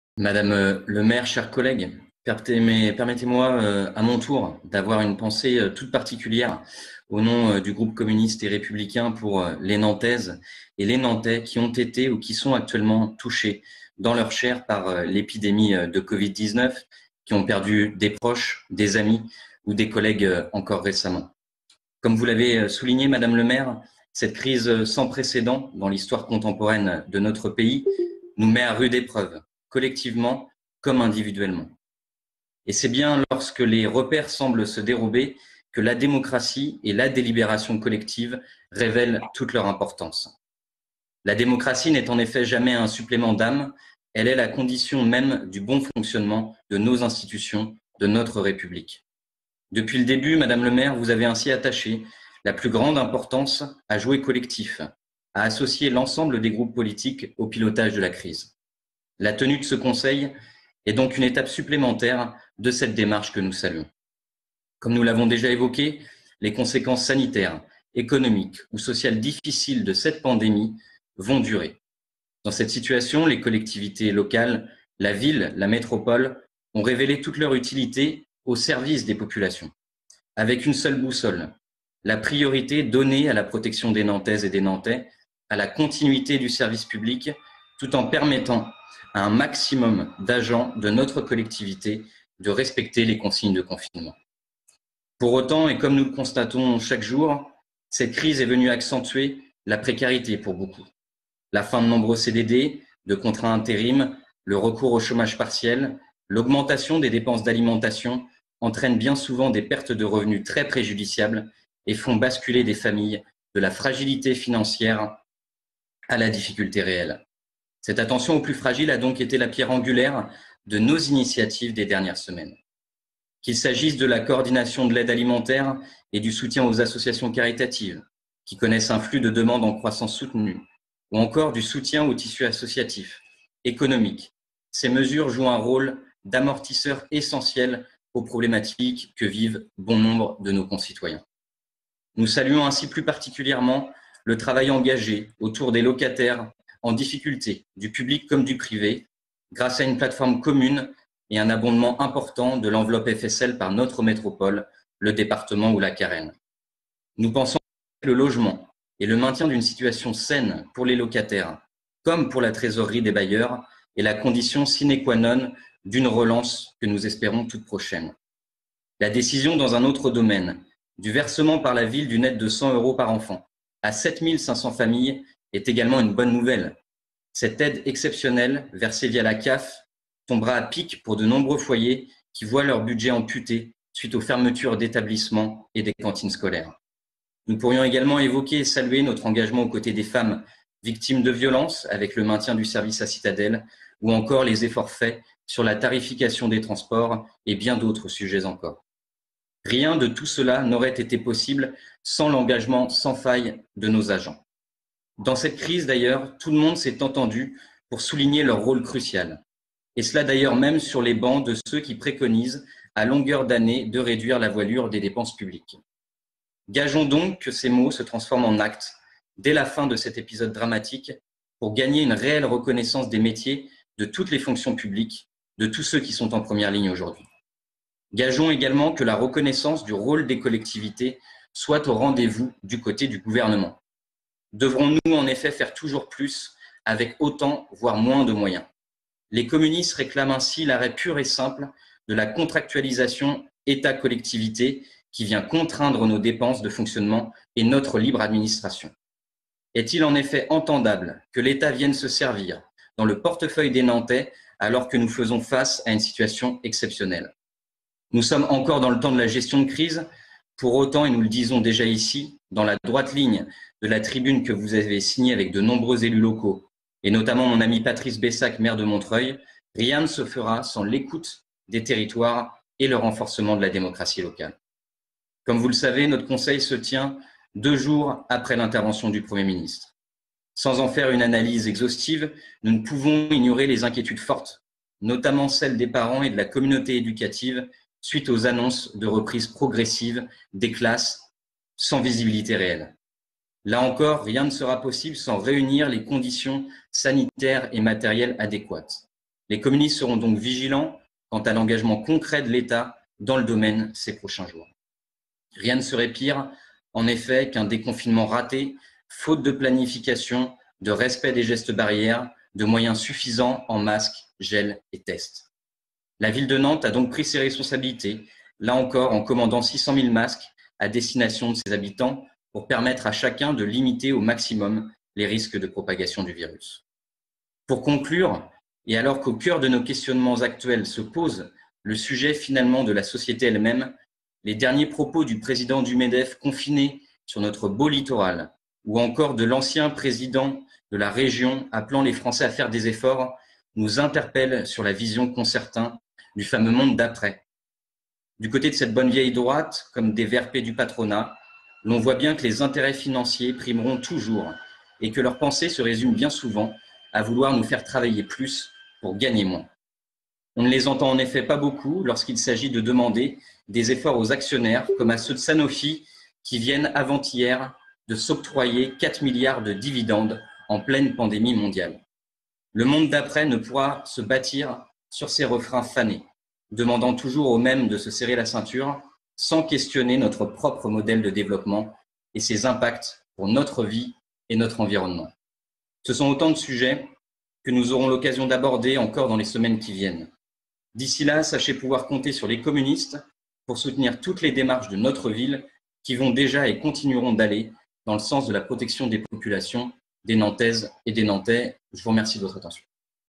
Situation sanitaire et sociale : Robin Salecroix au conseil municipal de Nantes du 30 avril 2020
Intervention de Robin Salecroix au conseil municipal de Nantes du 30 avril 2020 sur l'échanges sur l’action de la ville pendant la crise sanitaire.